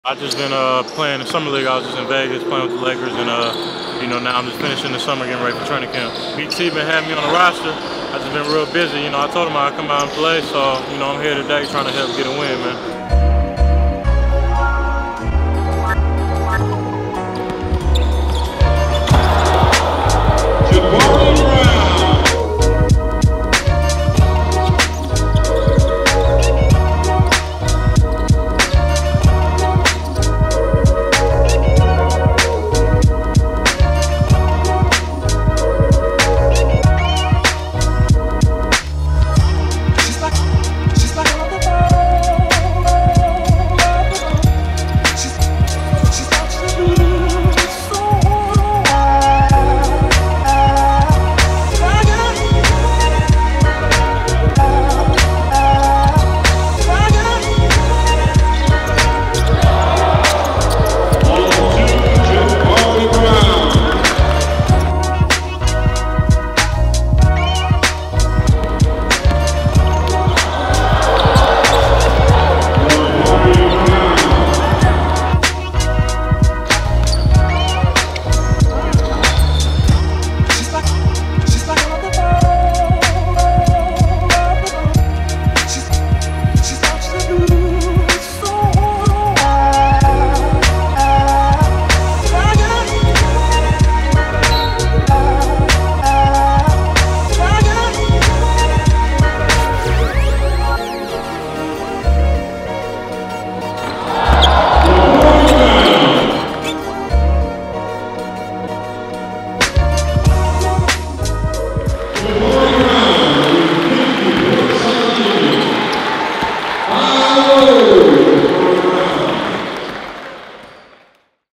Jabari Brown DROPS 50 POINTS at the SF Proam!!
***Make sure to watch in HD*** LA Lakers Guard and former Oakland Soldier, Jabari Brown made an appearance at the SF Proam last night and dropped 50 points while playing for the Bay Raiders. Check out some of the highlights here.